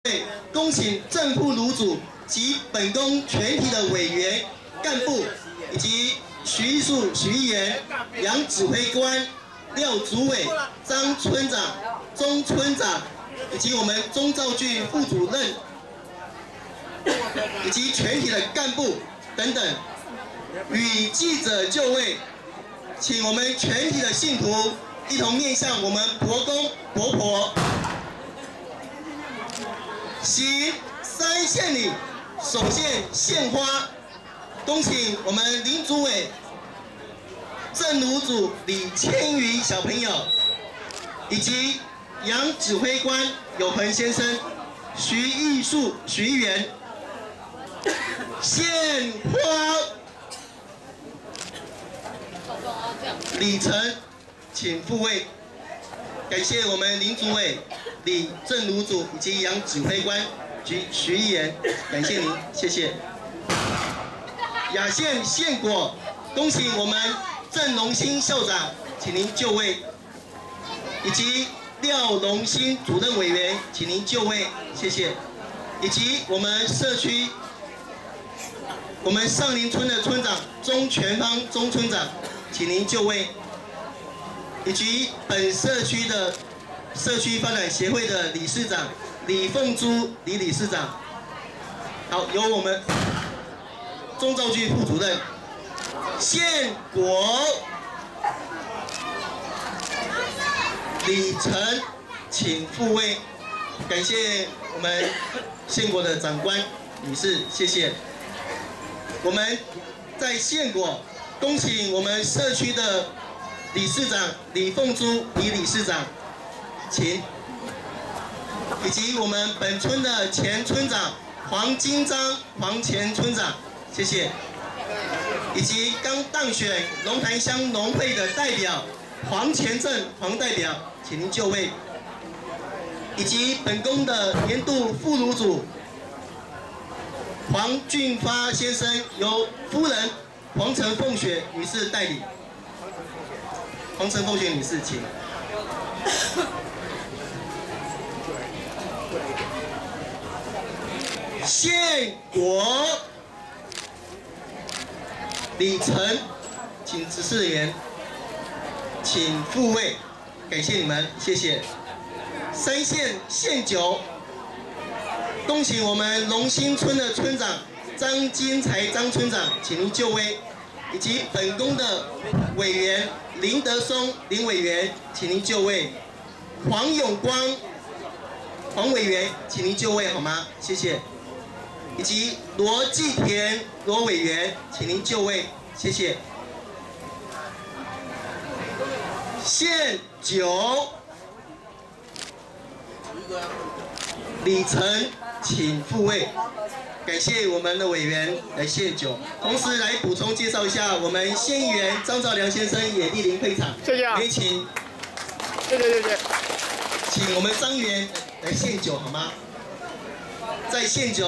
上林村福德宮102年伯公聖誕千秋01968
上林村福德宮(四方林開庄伯公)管理委員會舉行「102年伯公聖誕千秋」
地點:福德宮：上林村十鄰溝西30號對面
＊9:30貴賓致詞
＊10:30擲筶求龜
＊聯絡人：林昭賢總幹事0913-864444